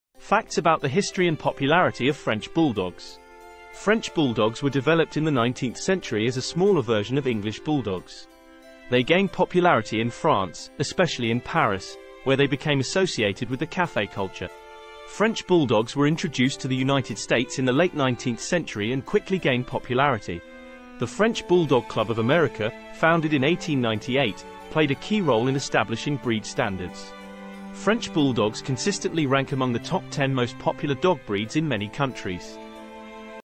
The History and Popularity of French Bulldogs!
Discover the origins of French Bulldogs, tracing their roots to 19th century England, where they were developed as a smaller version of the English Bulldog. We'll take you on a journey to France, where they gained immense popularity, particularly in the vibrant city of Paris. Uncover how they became an integral part of the Parisian cafe culture and the beloved companions of the working class and artists.